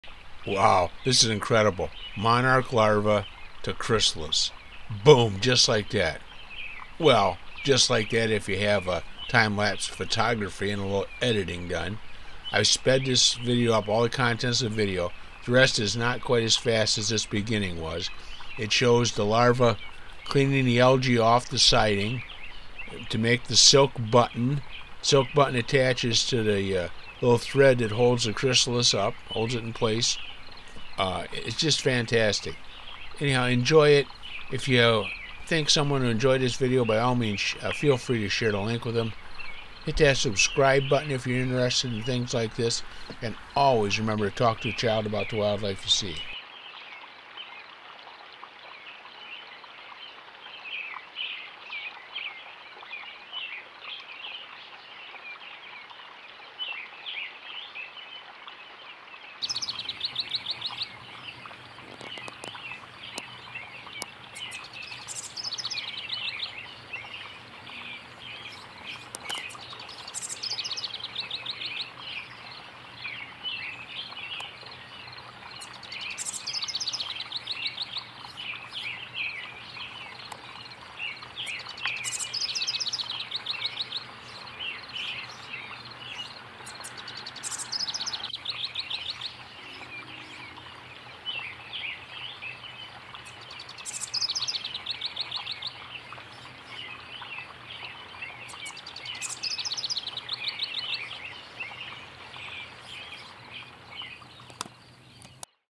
Monarch Larva to Chrysalis in 1 min, 46 seconds. Plus Making Silk Button
Slower Metamorphosis starts at 51seconds